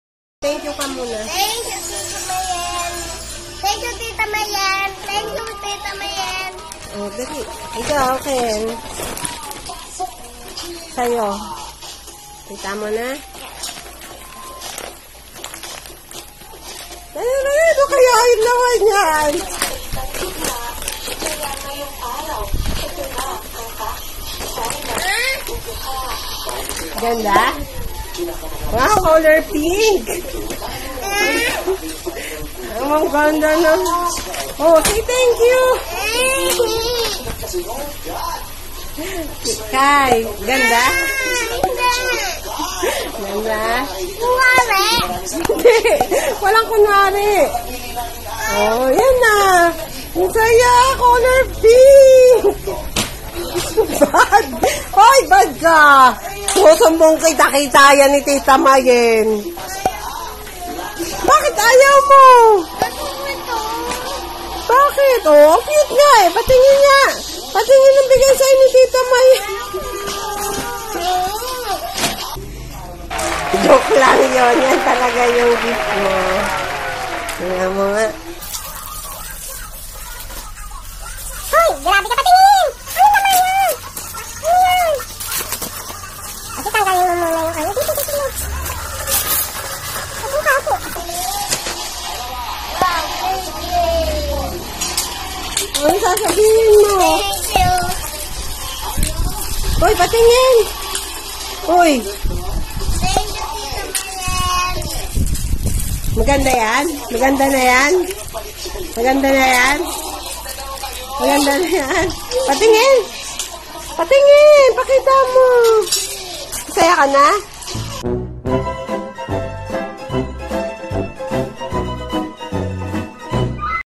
PRANK! Galit sa Pink! My Cute Pamangkins
We sent gifts to our pamangkins and we decided na lokohin si Kyrie na kunyari pink toy yung gift namin sa kanya 😂😂 watch till the end!